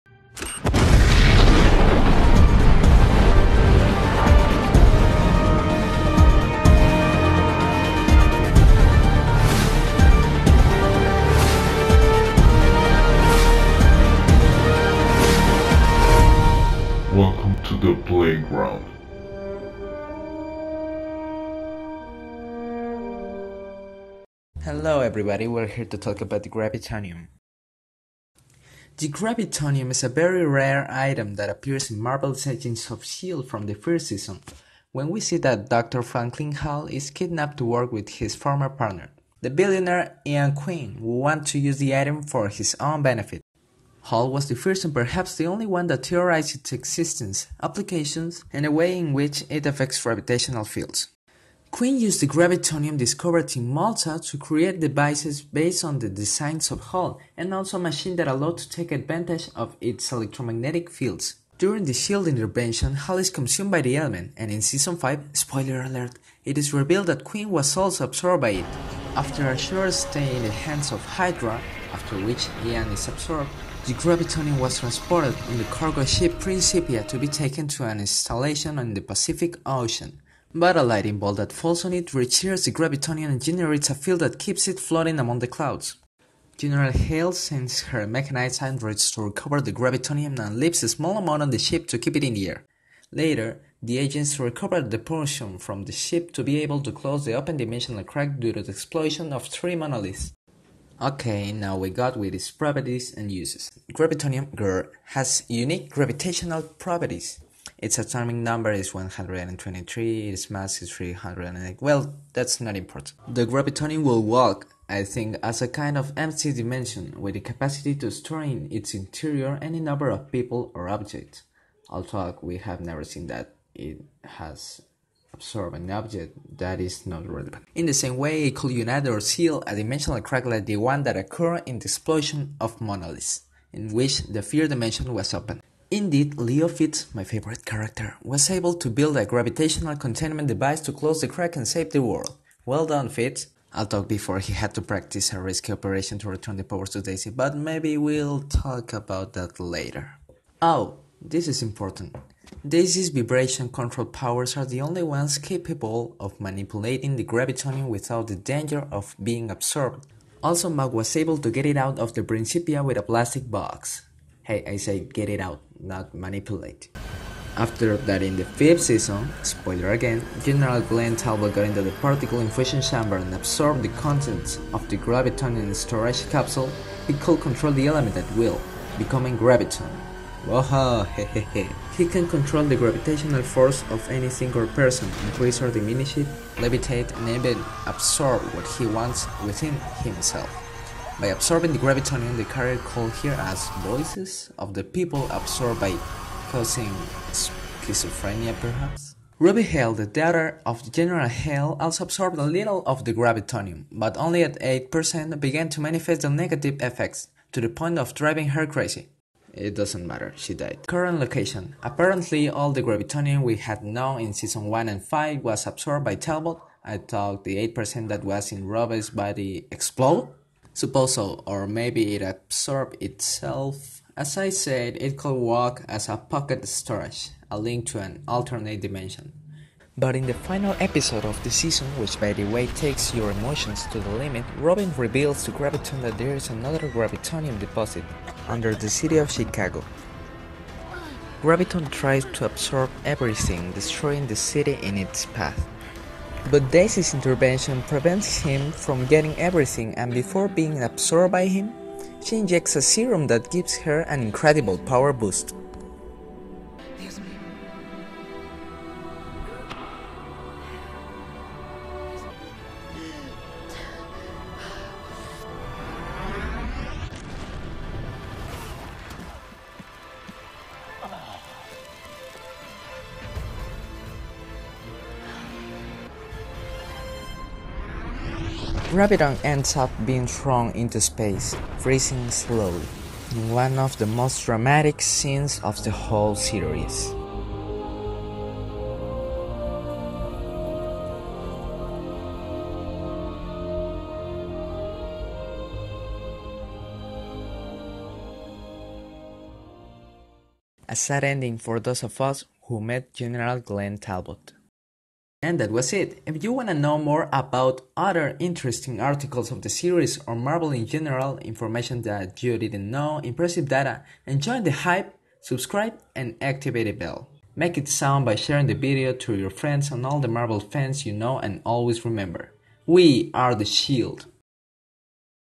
What is Gravitonium?? - Agents of SHIELD guide (season 5 spoilers!!)
Do you understand the nature of Gravitonium? In this video I explain everything you need to know. And some theories...
I hope you like the video =D
We are the SHIELD!!!